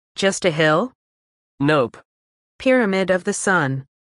Hidden Pyramid Revealed
How many large “hills,” like the one seen at Teotihuacan in 1900, are actually ancient pyramids waiting to be excavated today?

📷 unknown